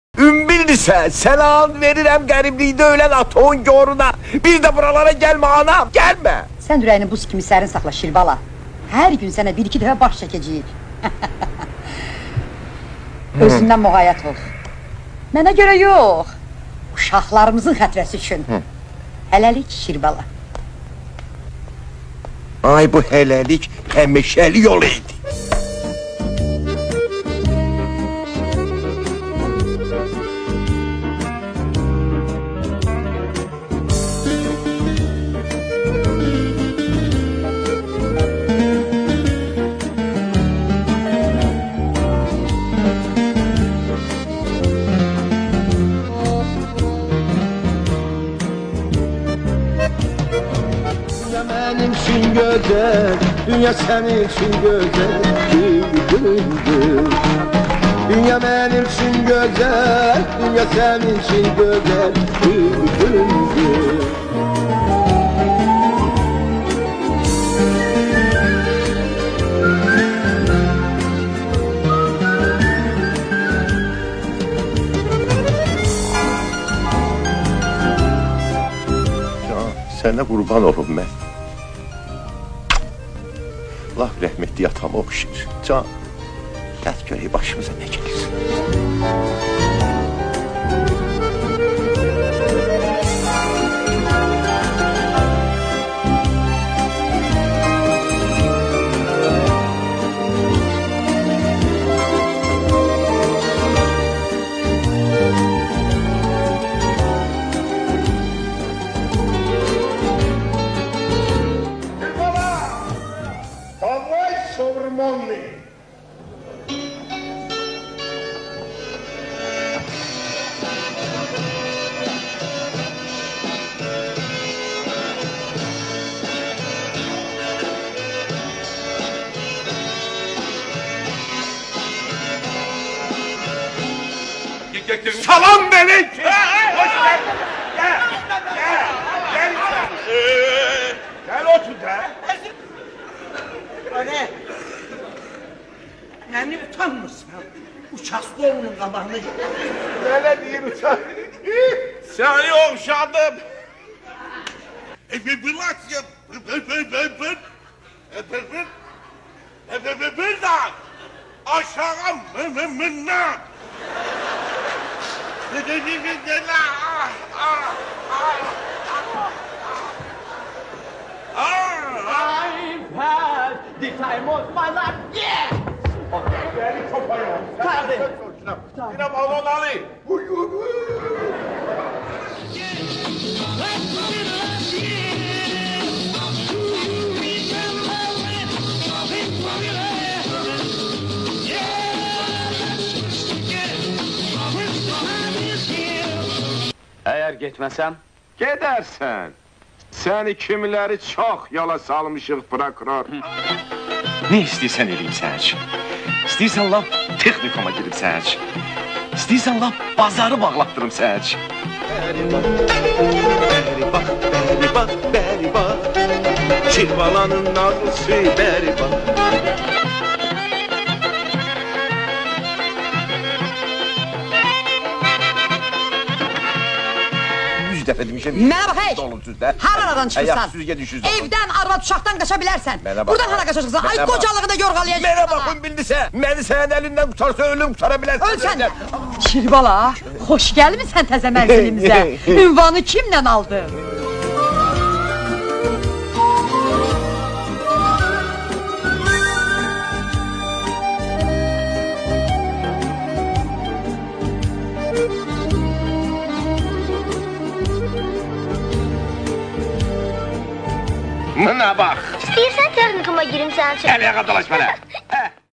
"Hicran" tamaşasında küsülü olan sənətkarlar...
"Hicran"da Mitoşbala, "Şirbalanın məhəbbəti"ndə Şirbala, "Alma almaya bənzər"də Məmmədəli
obrazları ilə yaddaşlara həkk olunan Hacıbaba Bağırov.
"Hicran" tamaşası çəkilən vaxt Hacıbaba Bağırovla Səyavuş Aslan küsülü idi